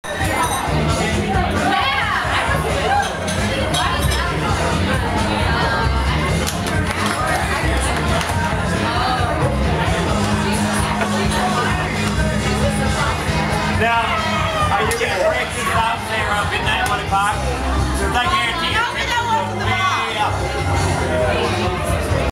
Dancing Bartenders
Two Bartenders At Casy Moran's in Chicago show off their routine